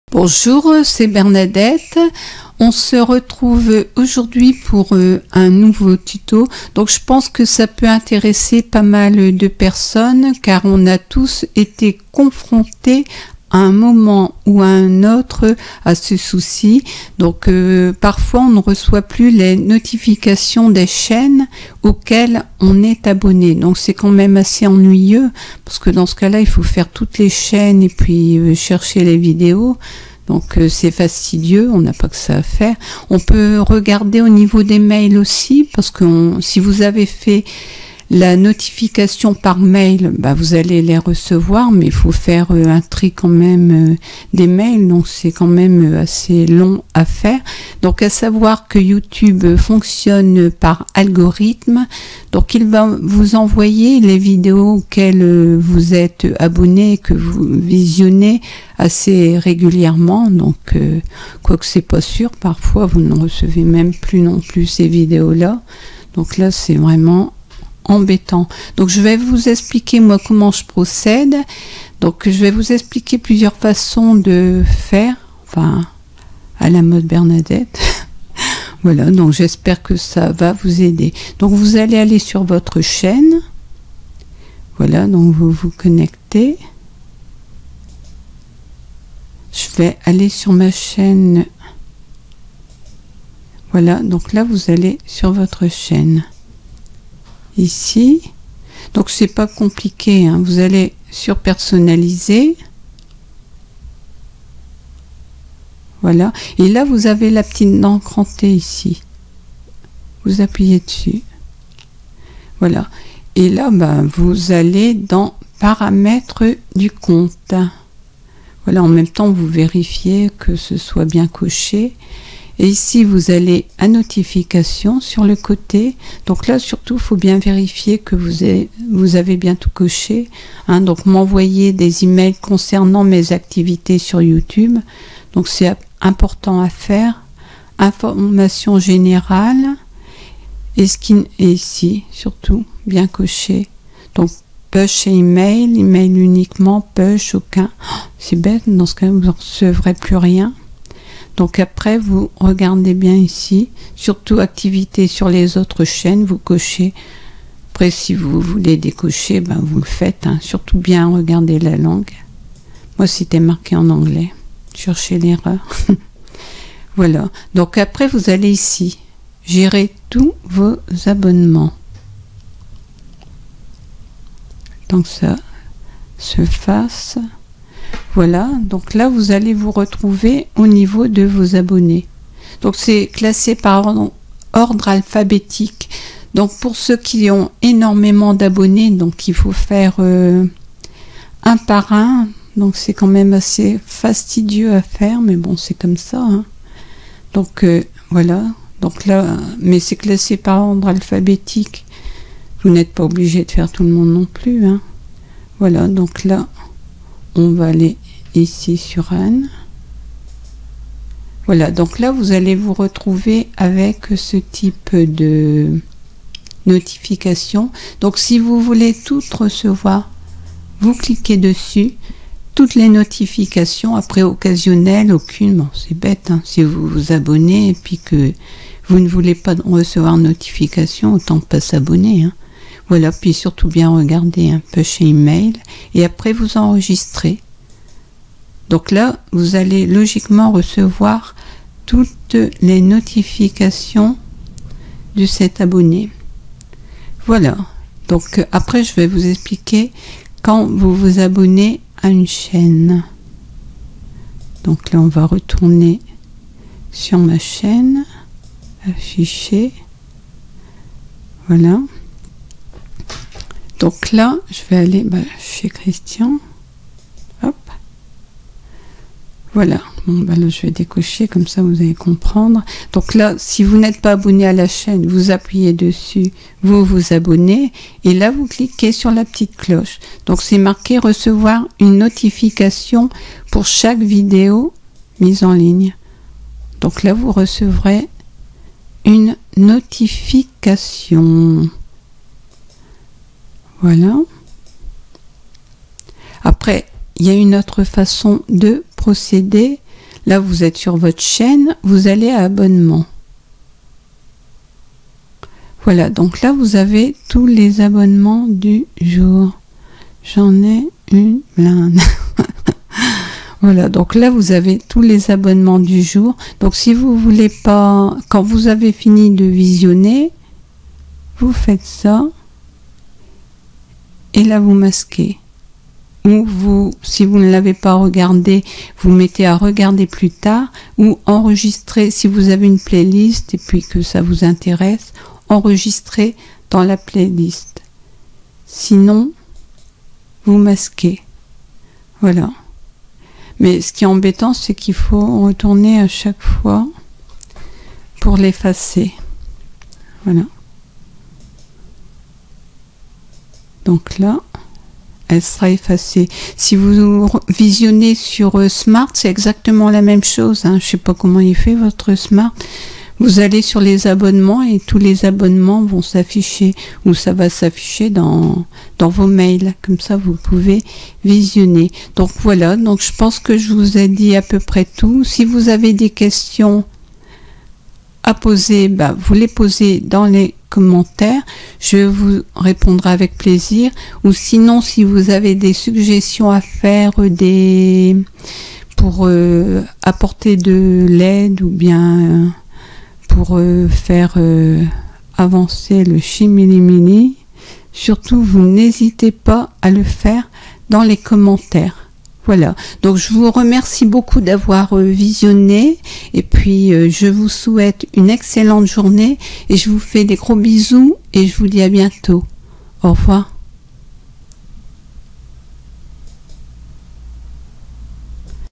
comment résoudre les bugs de réception des notifications vidéos sur YT
comment résoudre les bugs de réception des vidéos à laquelles vous êtes abonné(e)s

J,espére que cette vidéo vous sera utile

Si cela ne fonctionne toujours pas.
Mettez votre navigateur à jour ,  videz le cache de l'ordinateur ou changez de navigateur, moi perso j'ai Google Chrome et pas de soucis avec celui-ci

Si vous avez des questions des suggestions à faire n'hésitez pas à les faire dans les commentaires , j'y répondrai dans la mesure de mes connaissances .

Je vous souhaite une belle journée

Bisous Amitiés

Bernadette